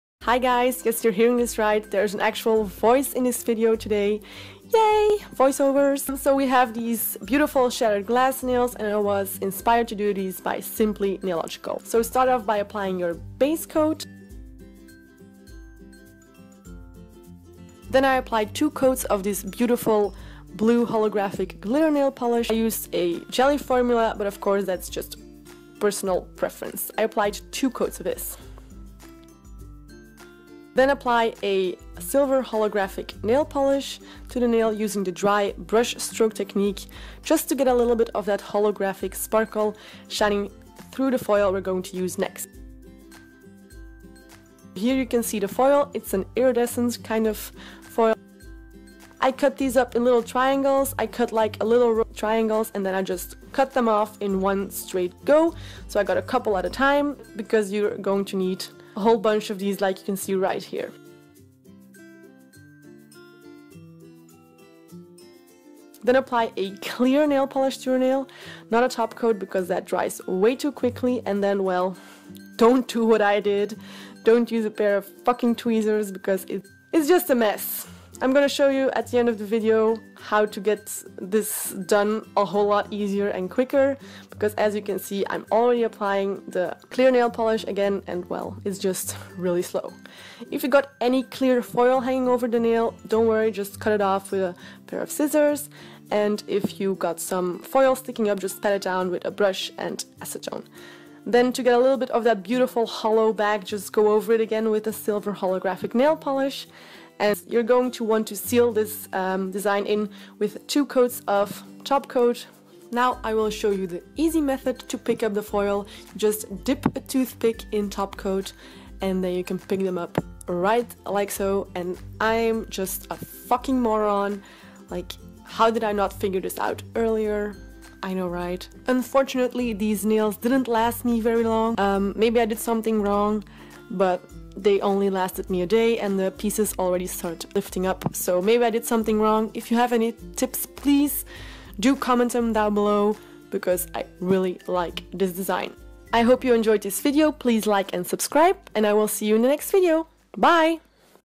Holographic Shattered Glass Nails | PaintedByPolish
Hi everyone! Omg this is so weird but this is my first voice over! It is FAR from perfect, I know that, but it is so much harder than it seems! Well atleast for me it was pretty hard, English is not my first language so sometimes I was not able to form my sentences correctly … hehe but it will get better I promise! ;) Also I recorded this with my IPhone so the quality is not great ;)

So I was inspired to do these nails by the AMAZING Simplynailogical! LOVE HER! Let me know how I did ;) I love how they turned out! Unfortunately they didn’t last very long, maybe I did something wrong, idk, let me know! ;)

xoxo Julie

Here are the products I used:

Iridescent foil (amazon link)

(I got my iridescent foil at a Belgian webshop because amazon does not deliver in my country so the link to that